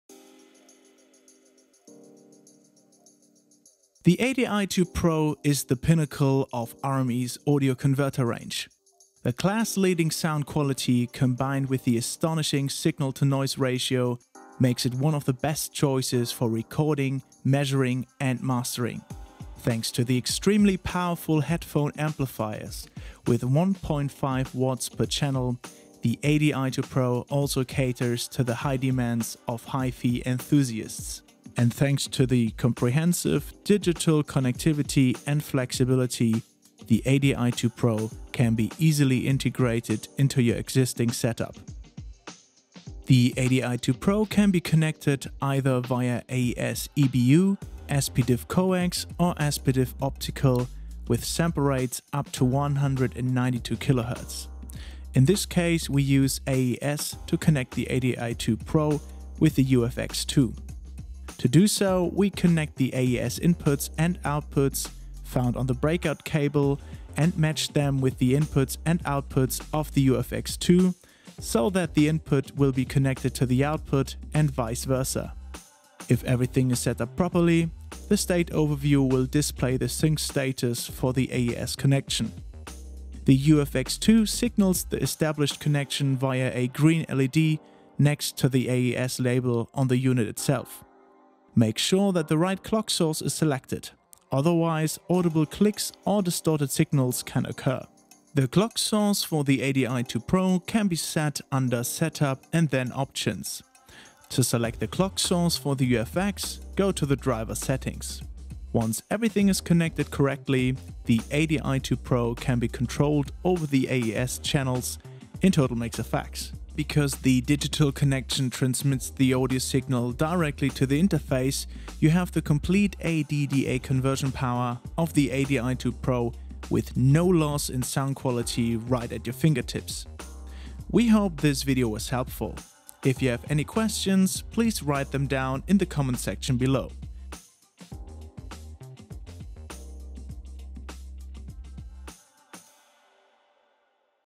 How to connect the ADI-2 Pro and TotalMix FX
The ADI-2 Pro is the pinnacle of RME's audio converter range. A class-leading interface with an astonishing signal-to-noise ratio makes it one of the best choices for recording, measuring and mastering.

The ADI-2 Pro can be connected either via AES/EBU, SPDIF coax or SPDIF optical with Sample rates up to 192 kHz. In this case we use AES to connect the the ADI-2 Pro with the Fireface UFX II.

Once everything is connected correctly, the ADI-2 Pro can be controlled over the AES channels in TotalMix FX. Furthermore, because the digital connection transmits the audio signal directly to the interface you have the complete AD/DA conversion power of the ADI-2 Pro with no loss in sound
quality right at your fingertips.